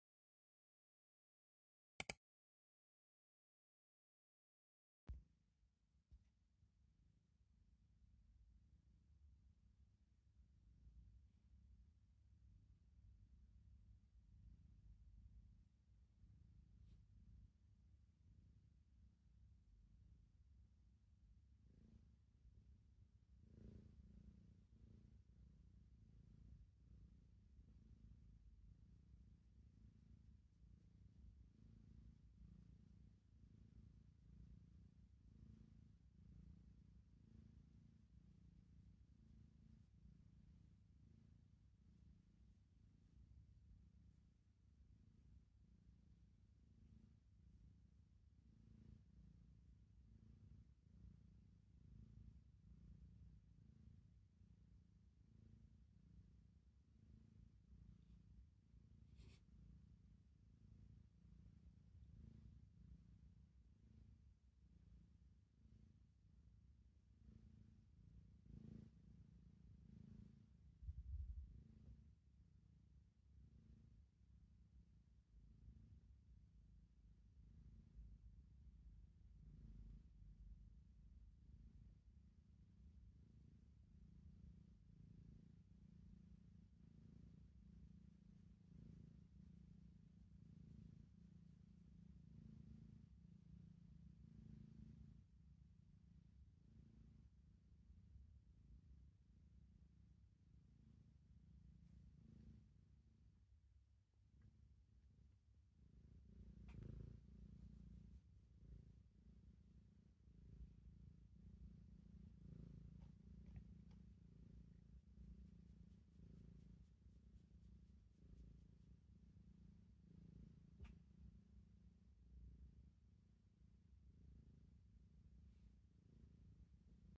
The cat is warm【Stella】
Hello.
Stella is warming up on my lap. I, too, am glad to be warm. Stella has recently started to ride on my lap😸